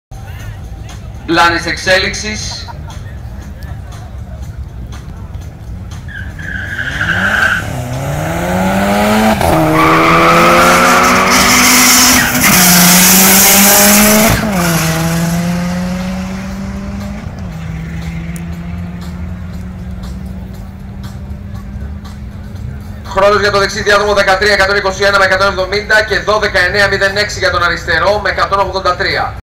0-402m : Octavia (Ntafos) 12.906@183 km/h - Sciroco (Aleksopoulos) 13.121@170 km/h
Thiva Drag Battle III - 19 & 20 October 2013